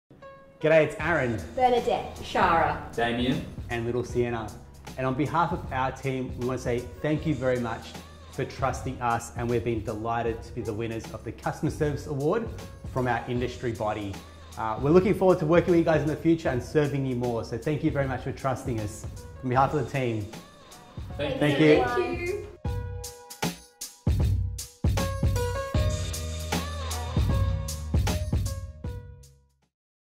Best Customer Service Award | MFAA
We’re absolutely pumped to have been recognised by our industry body, the Mortgage and Finance Association of Australia (MFAA) as the winner for the “Best Customer Service” in NSW. This is an incredible achievement for us and a testament to our clients who put their trust in us to help with their mortgages and investment loans.

Thank you to our partners who make this possible and especially to our talented team who go the extra mile to make the home loan experience memorable.